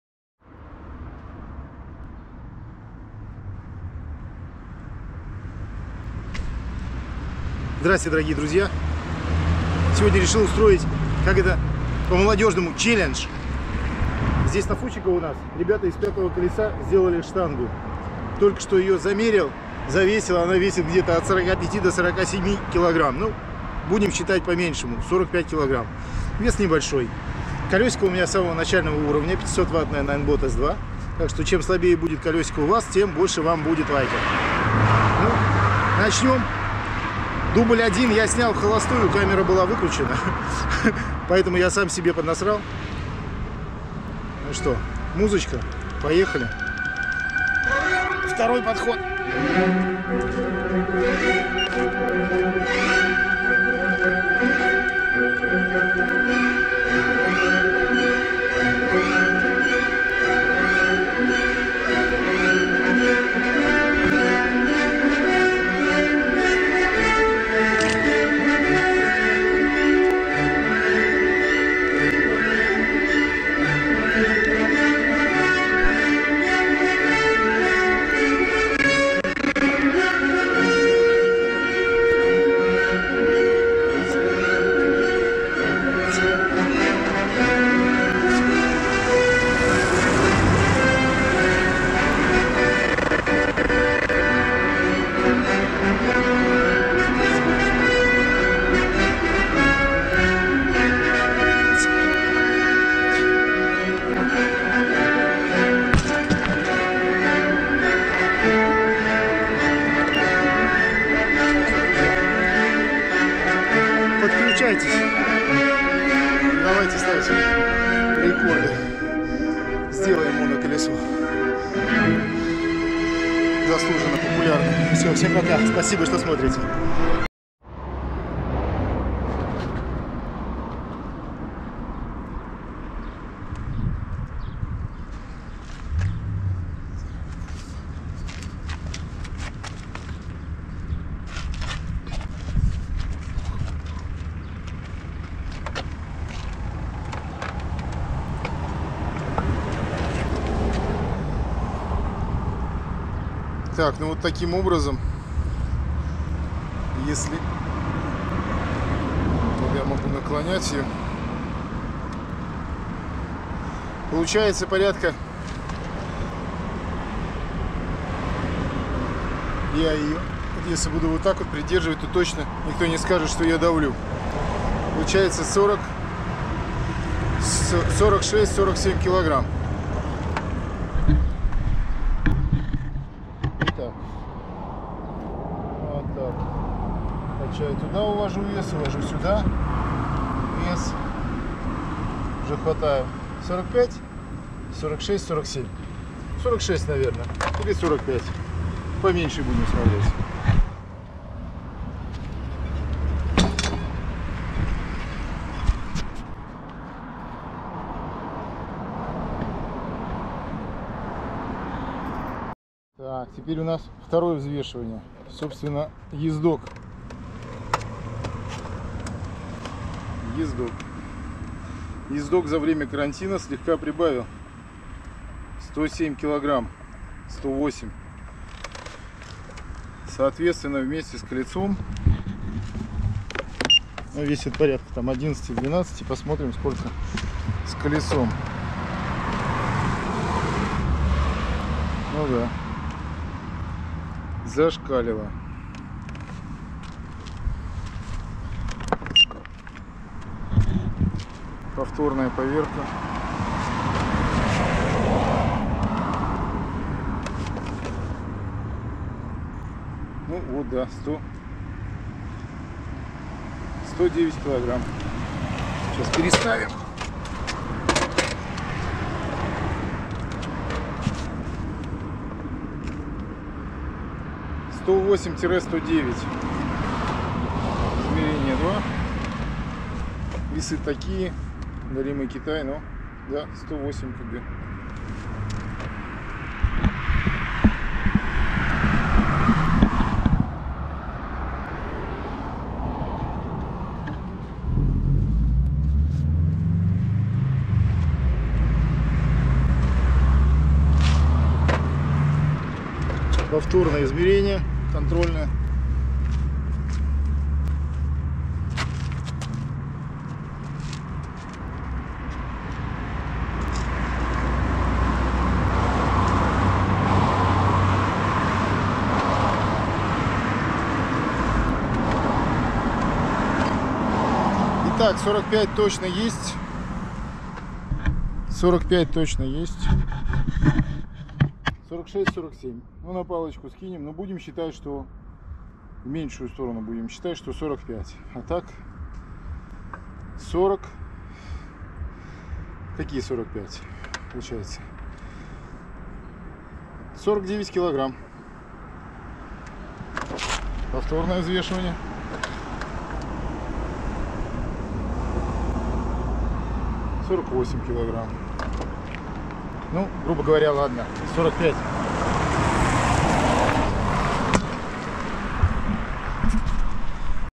Челлендж №1! Моноколесо и Штанга. Штанга 50 килограмм и моноколесо первого уровня ninebot S 2.
Данный челлендж только для сознательных, совершеннолетних, психически и морально устойчивых личностей! Челлендж, относится ко всем видам транспорта. Цель - показать превосходство моноколеса, перед всеми существующими персональными транспортными средствами. Моноколесо - самое совершенное, на данный момент персональное транспортное средство! Всем Добра! Очень приятно, что все дизлайки подкреплены комментариями! Дай Вам Бог Здоровья!